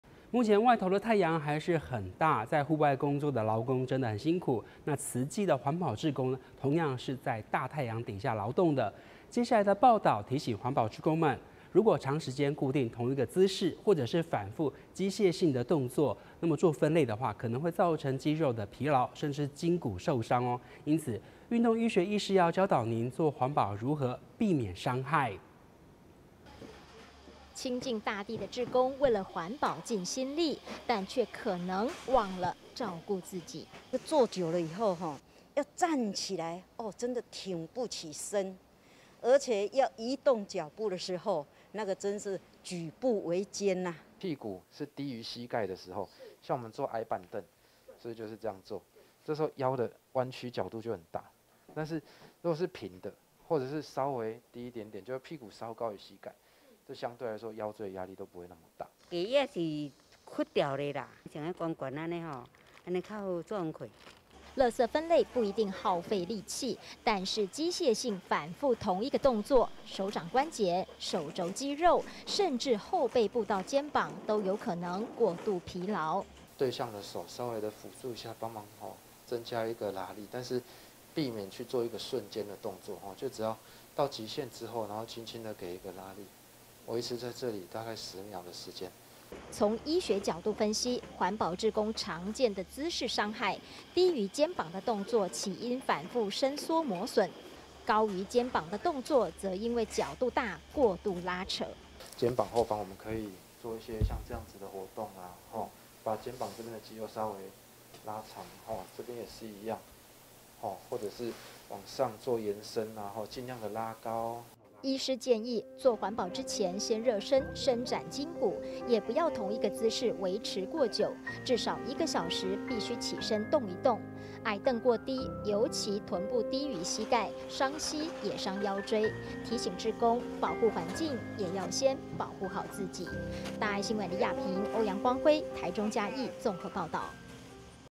坐矮凳反覆撿回收 小心傷害膝蓋腰椎｜大愛新聞
天氣熱，在戶外工作的勞工很辛苦，慈濟的環保志工，同樣也是在大太下勞動，接下來的的報導，提醒環保志工們，如果長時間固定同一個姿勢，或者反覆機械性的動作，做分類，可能造成肌肉疲勞、甚至筋骨受傷，因此運動醫學醫師要教導您，做環保如何避免傷害。